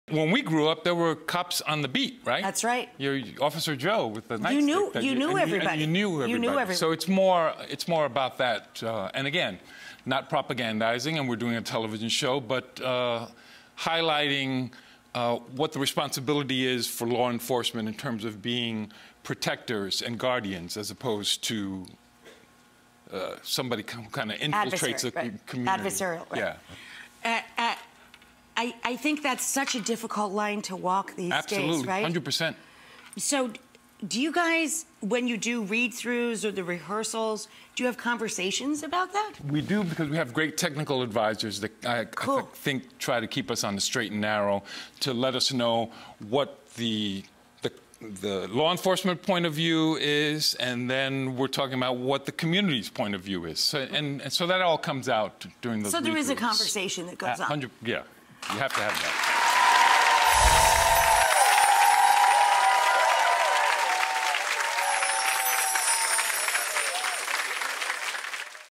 Jimmy Smits on New Police Drama "East New York"
Actor Jimmy Smits explains what it's like working on his new police drama "East New York."